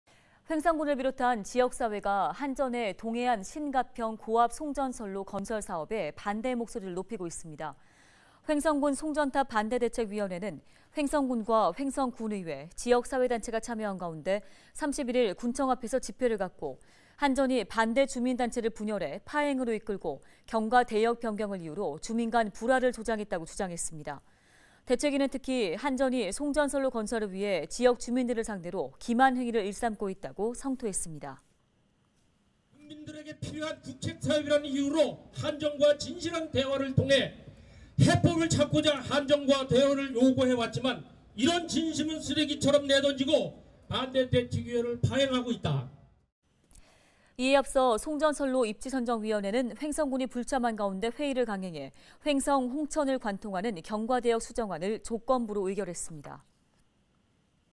횡성 동해안-신가평 송전선로 반대 집회
횡성군을 비롯한 지역사회가
한전의 동해안 신가평 고압 송전선로
건설 사업에 반대 목소리를
높이고 있습니다.
[헬로tv뉴스] 평일 오후 1시 30분/6시30분, 우리 동네 대표 뉴스
[제보] 카카오 ID ′LG헬로비전제보′ 검색